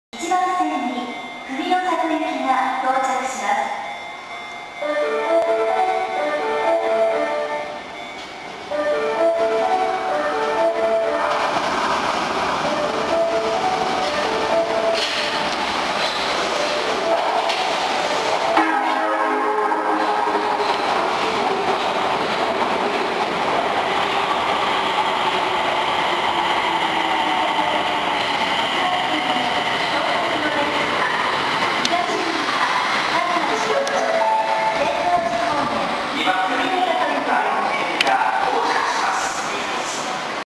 2017.07.10 大阪市営地下鉄 谷町線 30000系32610F 文の里行き到着 天神橋筋六丁目駅 大阪市交通局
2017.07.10 天神橋筋六丁目駅
32610F 6両編成
(Tec1)32610-(Mb1’)32110-(T')32810-(Mb2)32310-(Ma2)-32210-(Tec2)32910